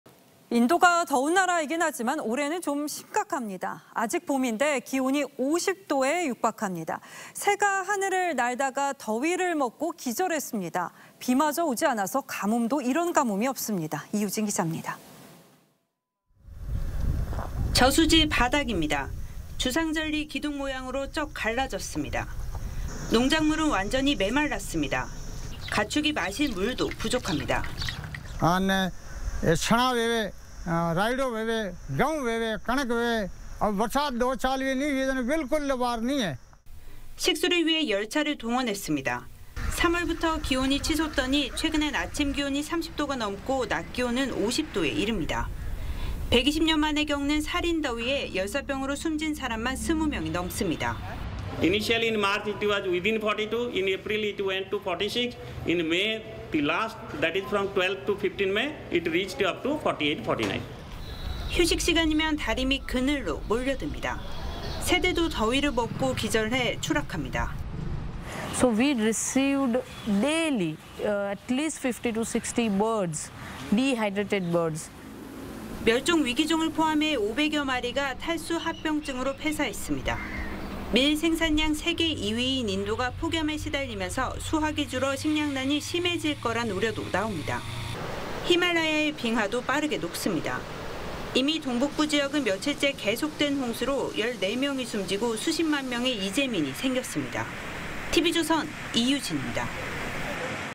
뉴델리 한낮 49도까지 올라…"날아가던 새도 더위에 떨어져"
인도가 더운 나라이긴 하지만, 올해는 좀 심각합니다. 아직 봄인데 기온이 50도에 육박합니다. 새가 하늘을 날다가 더위를 먹고 기절했습니다. 비마저 오지 않아 가뭄도 이런 가뭄이 없습니다.